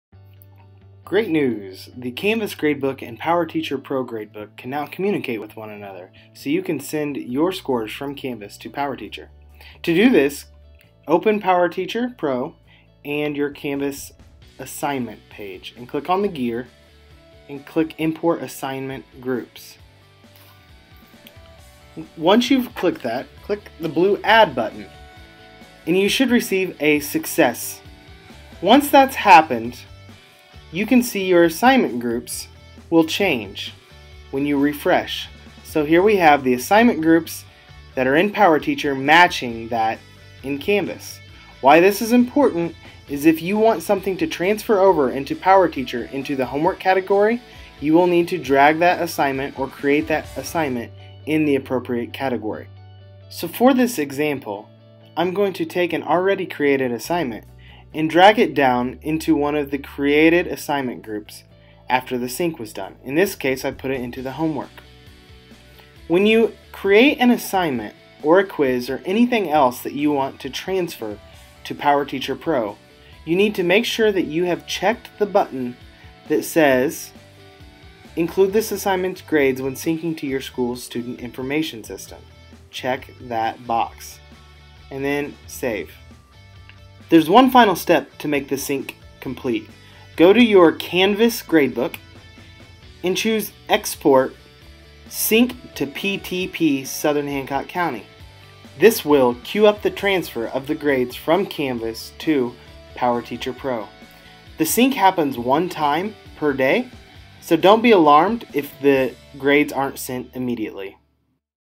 Canvas and PowerTeacher Pro Gradebook
How to get PowerTeacher Pro and Canvas to talk